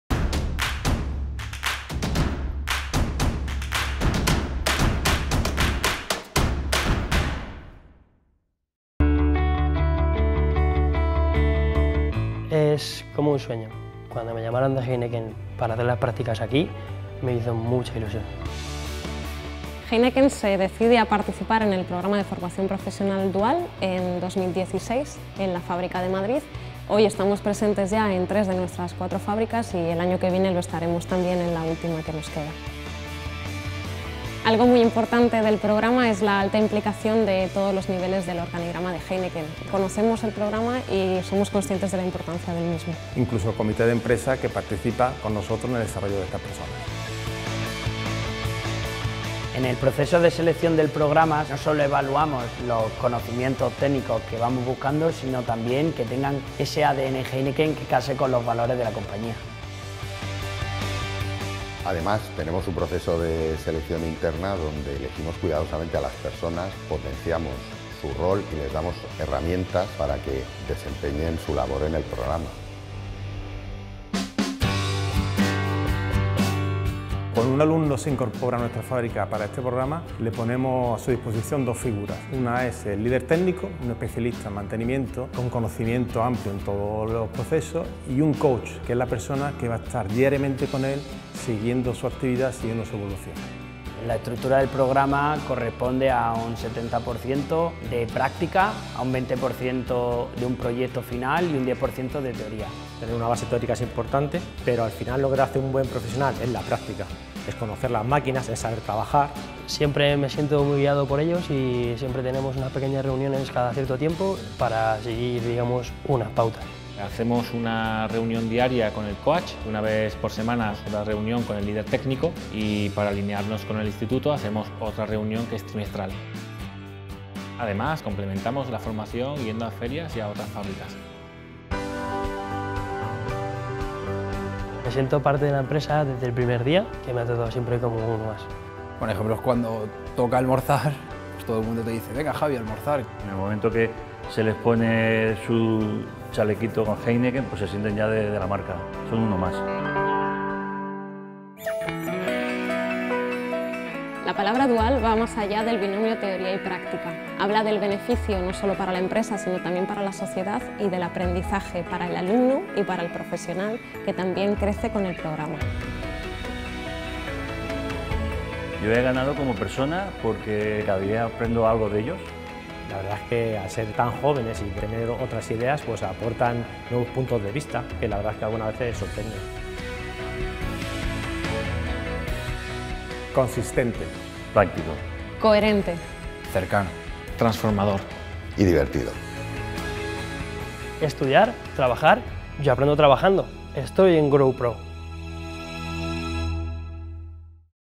"GrowPRO!" Heineken | I Premio Alianza
"GrowPRO!" de Heineken, proyecto ganador en la categoría "Gran Empresa" del I Premio Alianza para la FP Dual